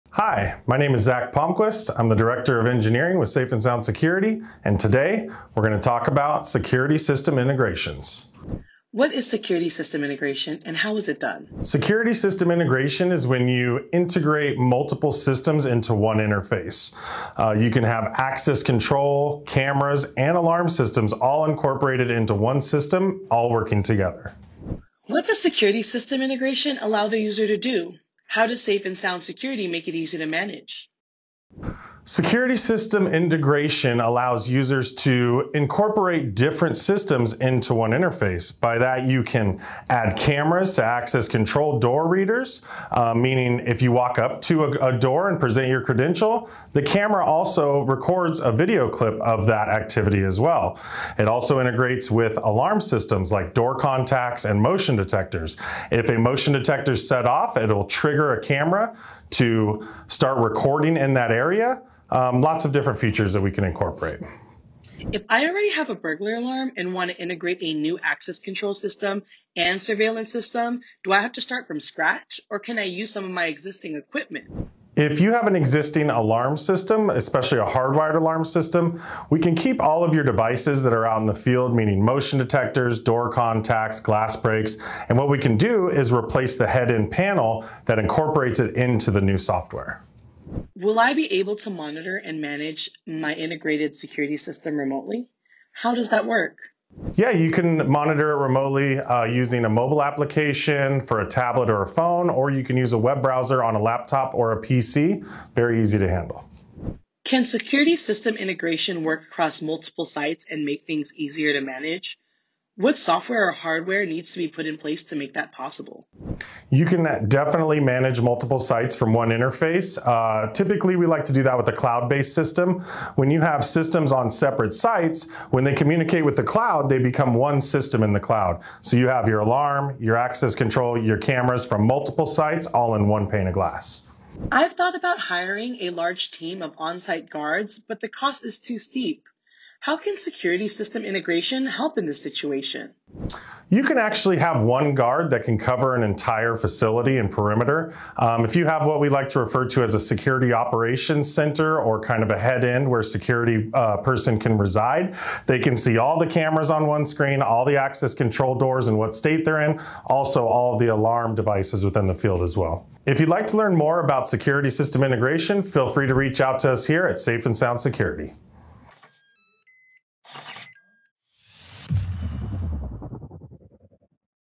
Security System Integration: Everything You Need To Know Before Installing
In this video, we'll be answering some of the most frequently asked questions about Security System Integration. This includes questions like: What is security system integration and how is it done?, What does security system integration allow the user to do? How does Safe and Sound Security make it easy to manage?, If I already have a burglar alarm and want to integrate it with a new access control system and surveillance system, do I have to start from scratch or can I use some of my existing equipment?, Will I be able to monitor and manage my integrated security system remotely? How does that work?, Can security system integration work across multiple sites and make things easier to manage? What software or hardware needs to be put in place to make that possible?, and I’ve thought about hiring a large team of on-site guards but the cost is too steep. How can security system integration help in this situation?. Keep watching to learn more and if you'd like Zac to answer your question in the next videos, please leave your question below!

Timestamps

00:00 Security System Integration Intro
00:08 What is security system integration and how is it done?
00:25 What does security system integration allow the user to do? How does Safe and Sound Security make it easy to manage?
01:05 If I already have a burglar alarm and want to integrate it with a new access control system and surveillance system, do I have to start from scratch or can I use some of my existing equipment?
01:30 Will I be able to monitor and manage my integrated security system remotely? How does that work?
01:47 Can security system integration work across multiple sites and make things easier to manage? What software or hardware needs to be put in place to make that possible?
02:14 I’ve thought about hiring a large team of on-site guards but the cost is too steep. How can security system integration help in this situation?
02:43 Conclusion